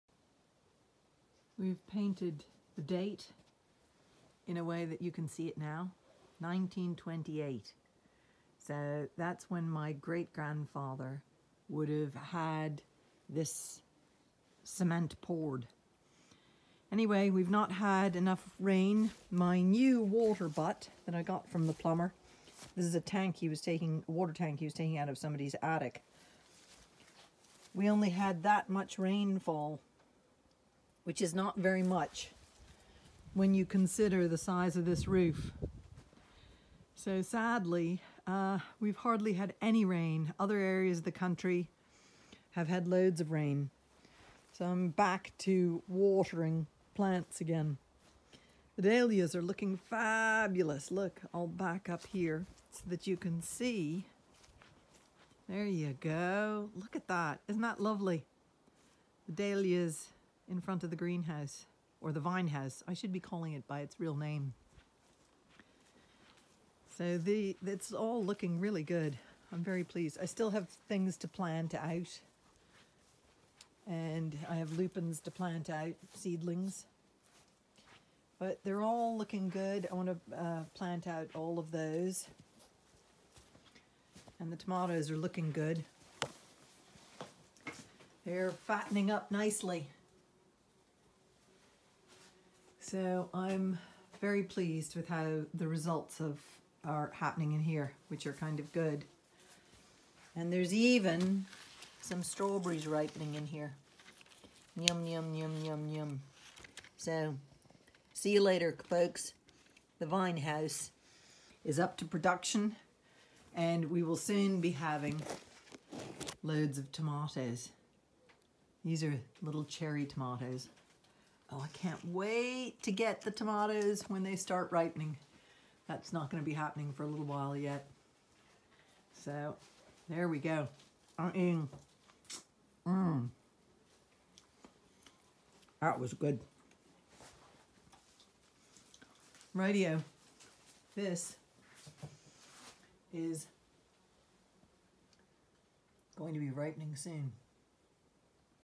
Vine house’s new water butt has hardly any water yet due to little rain
Slowly the vine house is getting up to speed. Soon we shall have ripening tomatoes. I’m so looking forward to next year when I can start things off when it’s still cold outside.